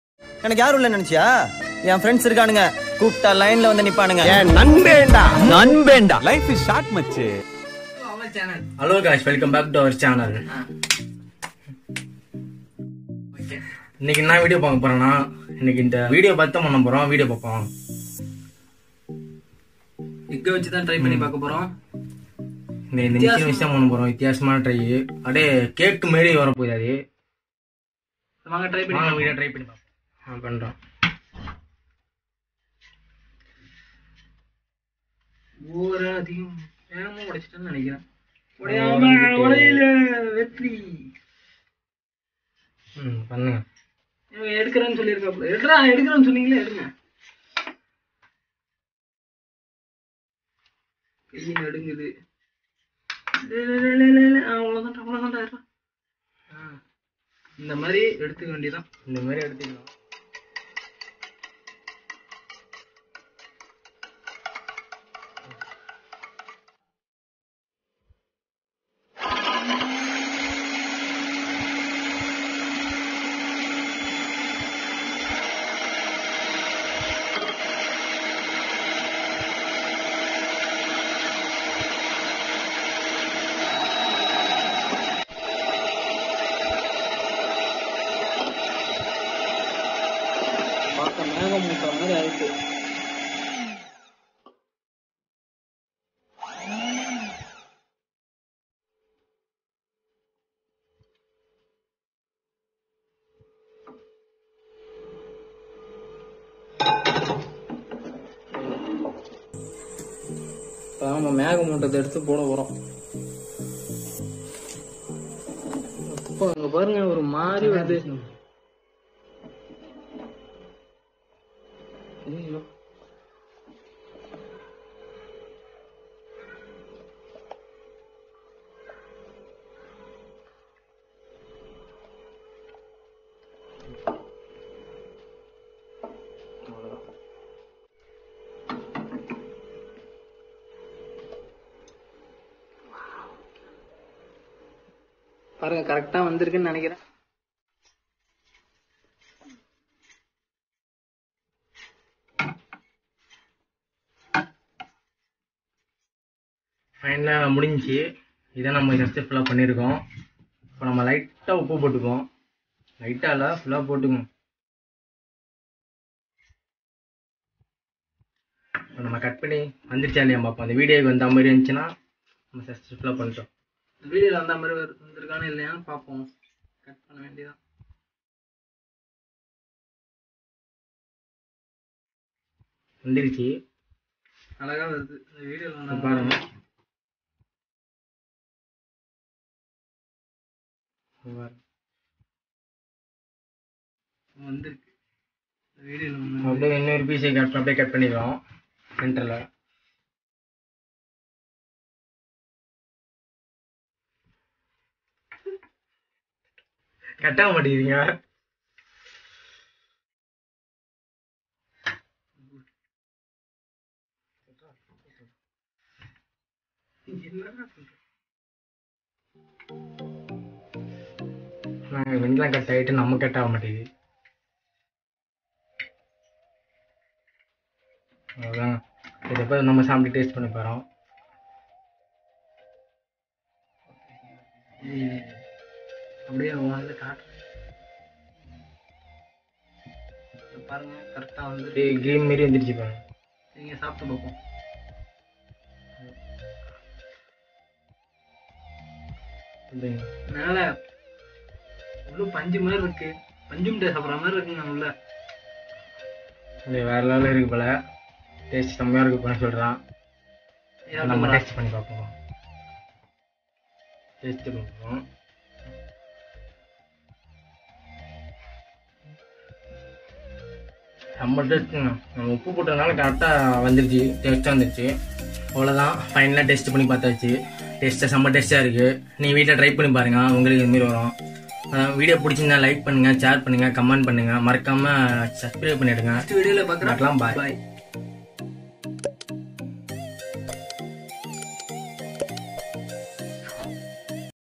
Testing Out Viral Giant Whipped Egg Pie By 5 Minutes Craft | Cooking Comali [TAMIL]
Today Iam Testing Out Viral Giant Whipped Egg Pie By 5 Minutes Craft